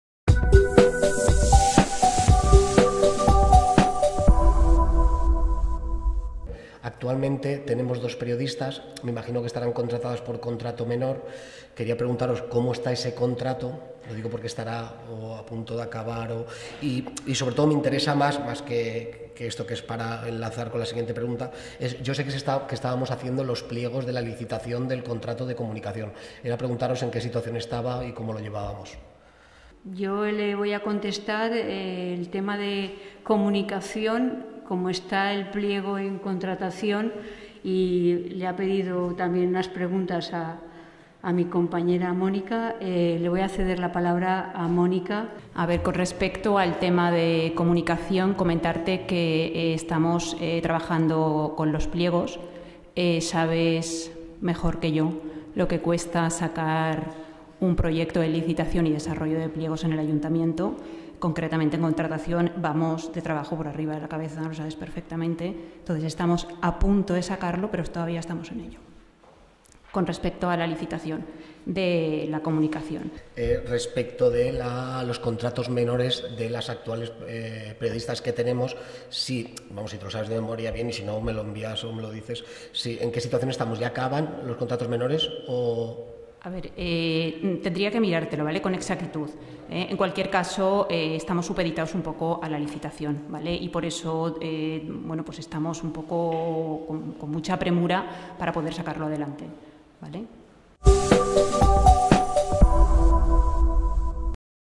1ª Pregunta de José Rodado. Ple d'octubre 2020. Contracte de Comunicació.
En el Ple Ordinari del mes d'octubre, celebrat aquest dilluns, el regidor de Compromís José Rodado va realitzar diferents preguntes en el torn de Precs i Preguntes. La primera d'elles va anar dirigida a la regidora municipal de Contractació, Mónica Martín. Així, Rodado li va preguntar pels plecs de la licitació del contracte de comunicació i en quina situació es trobava aquest.
L'edil responsable d'aquesta àrea li va indicar que, efectivament, estan treballant amb els plecs i estan a punt de traure'l a licitació.